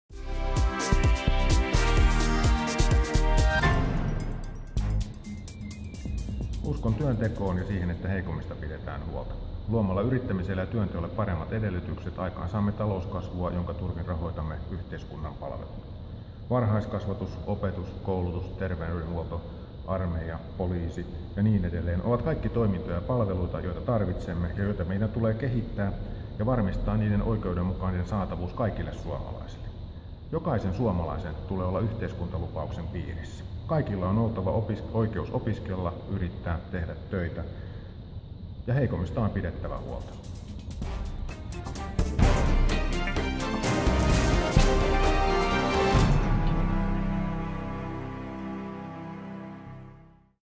Jukka Kopra - Paremmat edellytykset yrittämiselle ja työnteolle
Jukka Kopra puhuu yrittämisen, työnteon ja ihmisten hyvinvoinnin edellytyksistä. Jokaisen suomalaisen tulee olla yhteiskuntalupauksen piirissä.